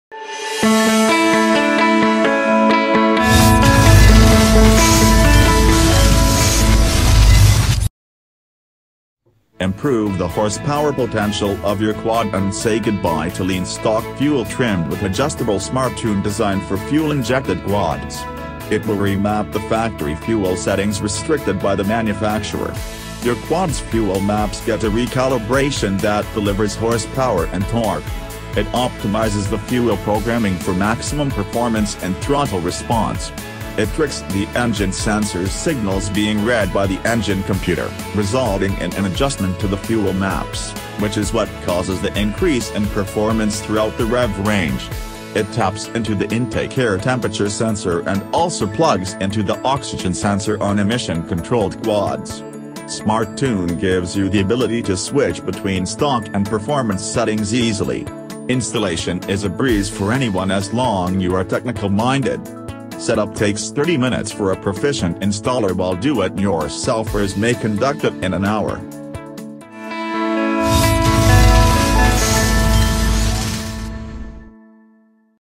Smart Tune ATV Fuel Tuner Aggressive Boost in PERFORMANCE!
Improve the horsepower potential of your quad and say goodbye to lean stock fuel trim with adjustable Smart-Tune designed for fuel injected quads. It will remap the factory fuel settings restricted by the manufacturer. Your quad’s fuel maps get a recalibration that delivers horsepower and torque. It optimizes the fuel programming for maximum performance and throttle response. It tricks the engine sensors’ signals being read by the Engine Computer, resulting in an adjustment to the fuel maps, which is what causes the increase in performance throughout the rev range. It taps into the Intake Air Temperature sensor and also plugs into the oxygen sensor on emission controlled quads. Smart-Tune gives you the ability to switch between stock and performance settings easily. Installation is a breeze for anyone as long you are technical minded. Setup takes thirty minutes for a proficient installer while do-it-yourselfers may conduct it in an hour.